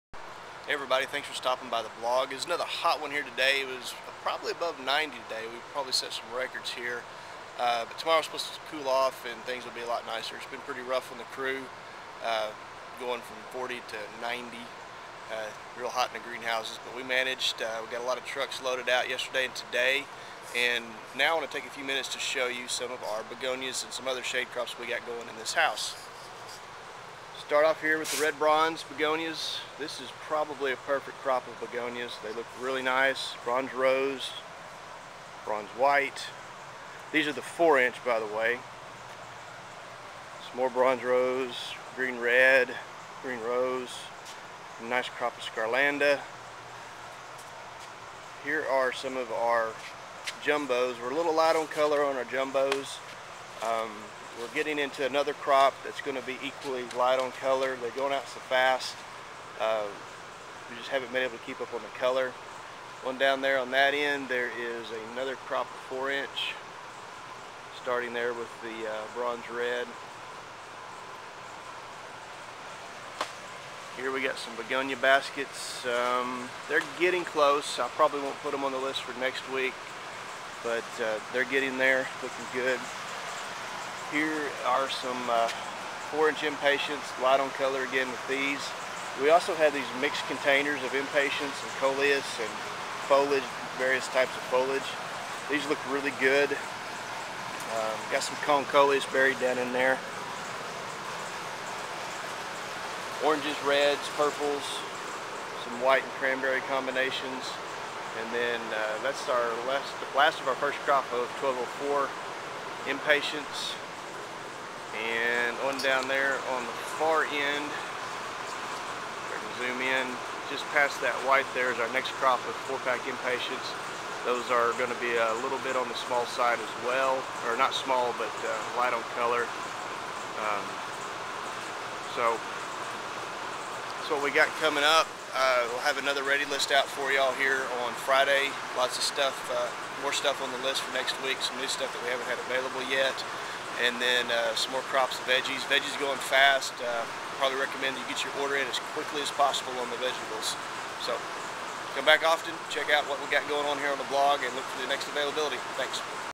begonias impatiens
March 19, 2009 - #4 and J6 Begonias, Begonia baskets, 1204 and #4 Impatiens and #12 Impatiens Combo Deco pots.